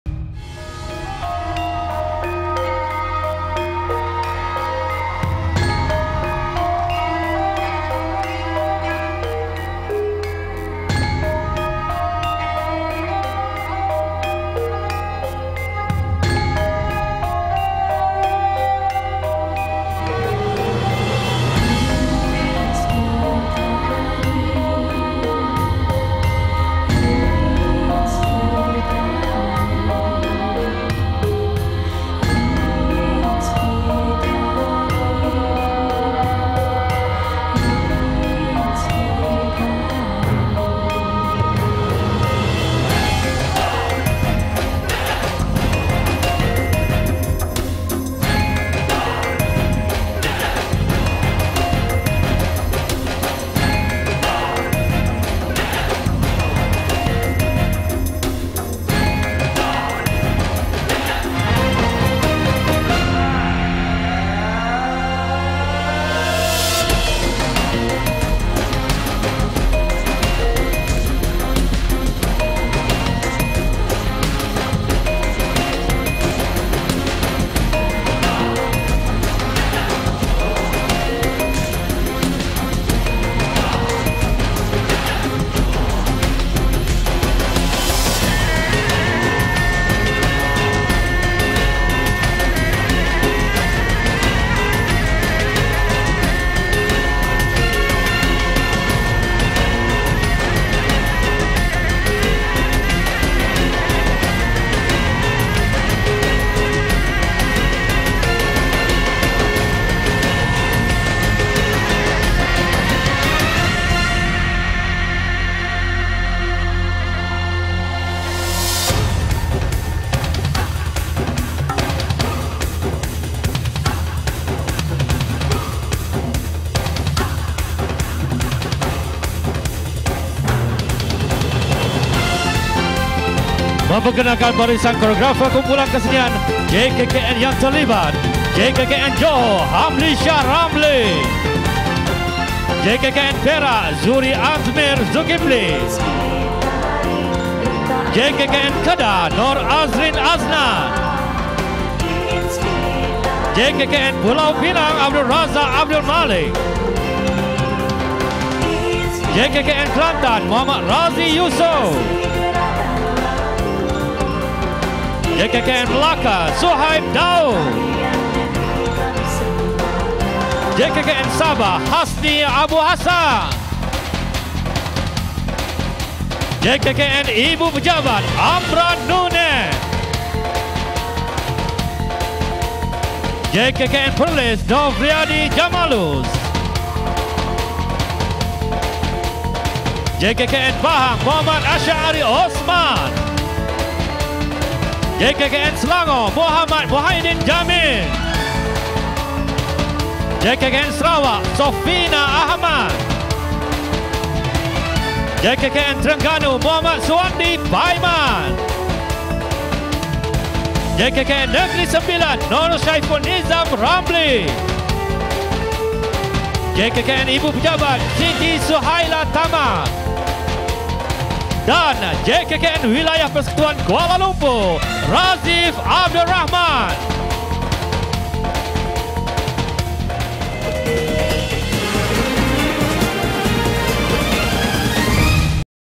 PERSEMBAHAN PEMBUKAAN INSPITARI 2014
Inspitari merupakan salah satu program penting Jabatan Kebudayaan dan Kesenian Negara dalam usaha memperkembangkan dan menyemarakkan seni tari di negara ini. Tahun 2014  merupakan tahun kelima  Inspitari dilaksanakan. Jika diimbas kembali sejarah  bermulanya  Inspitari  pada tahun 2010 hingga  2013, sejumlah 80 karya baharu telah berjaya dihasilkan   dengan  garapan  dan idea baharu, tetapi  masih  mengekalkan  ciri-ciri asal identiti Malaysia. Manakala pada tahun ini (2014) seramai  260  orang artis budaya Kumpulan Kesenian JKKN berserta 16 orang koreografer  akan menampilkan  16  karya baharu dalam program ini. JKKN juga telah mengambil inisiatif untuk mendokumentasikan karya baharu yang dihasilkan. Ia untuk dijadikan modul pembelajaran dan panduan kepada penggiat serta pelajar senitari di Malaysia. Sesungguhnya JKKN akan terus  komited dan gigih dalam  usaha  mencungkil dan menggali  khazanah  seni budaya negara yang terdapat di seluruh negara. Tujuannya adalah untuk diketengahkan dan seterusnya  dapat  dihayati bersama oleh segenap lapisan masyarakat Malaysia.

Semarakkan Seni dan Budaya Bangsa.